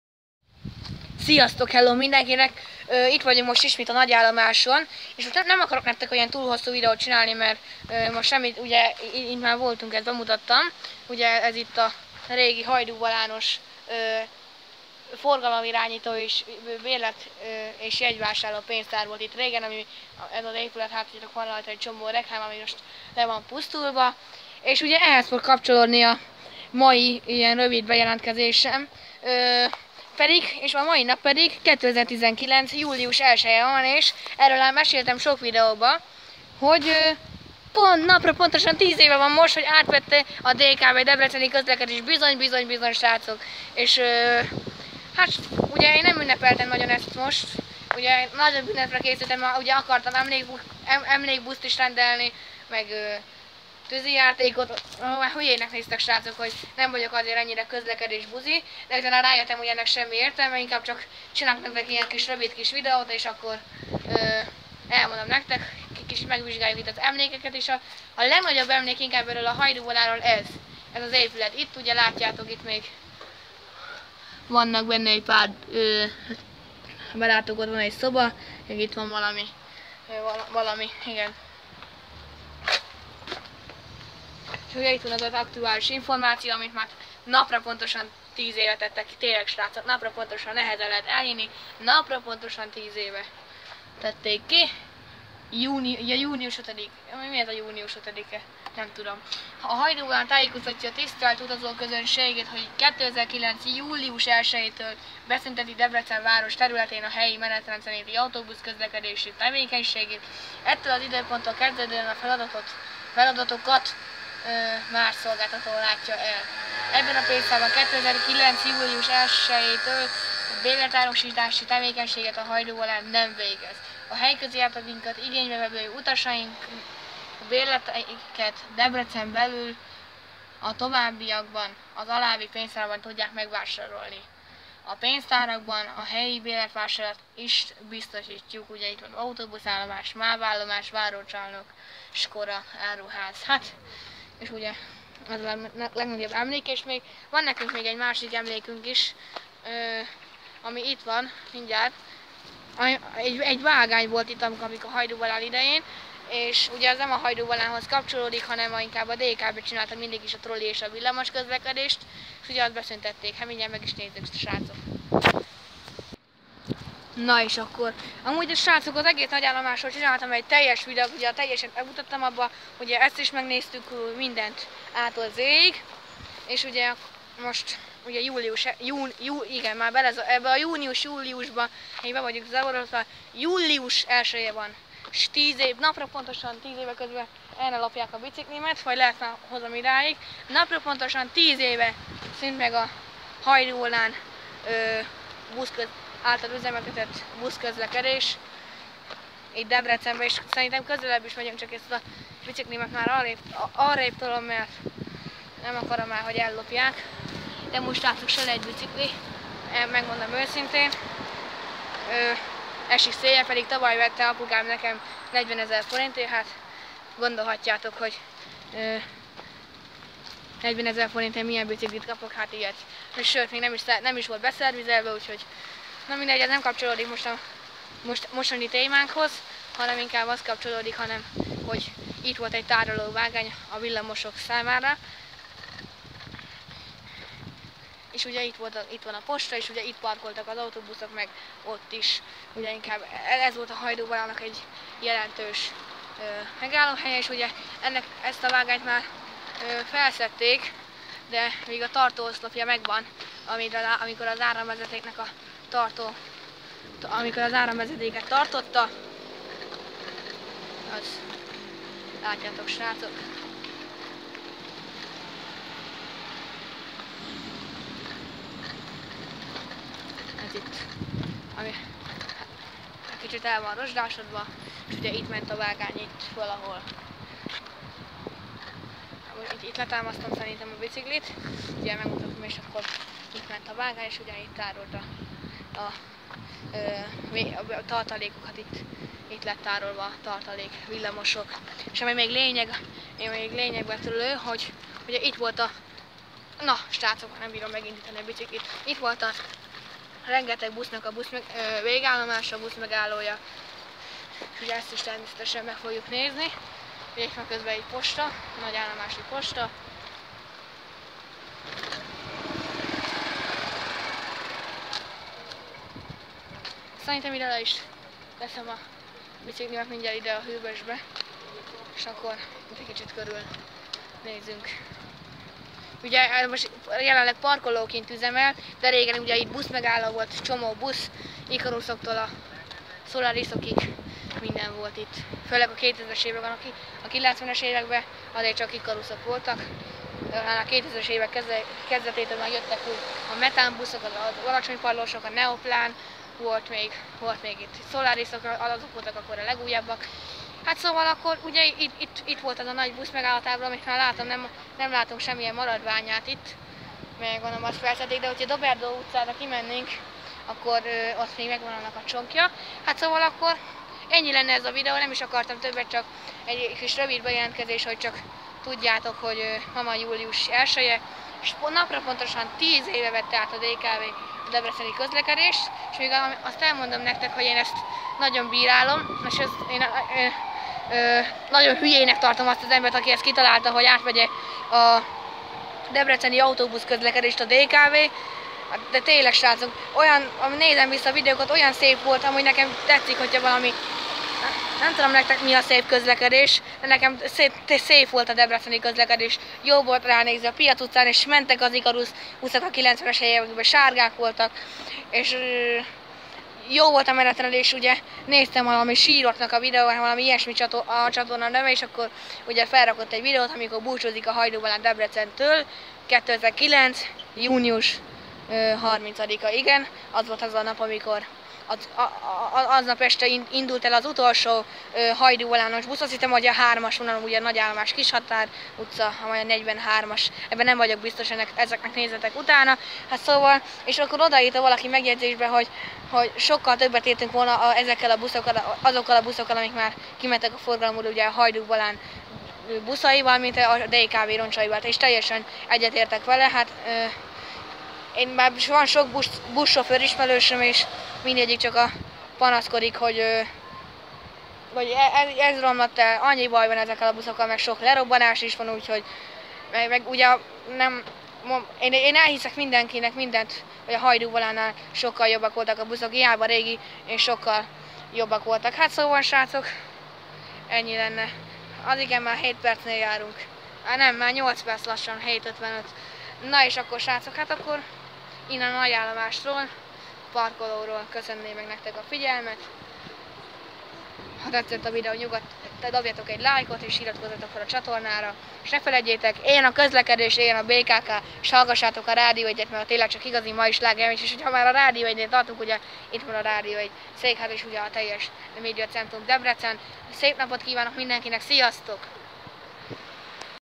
Jubileumi évforduló.Napra pontosan 10 éve vette át a DKV a debreceni buszközlekedést
Ma van július 1-je.10 éves a debreceni DKV-s buszközlekedés.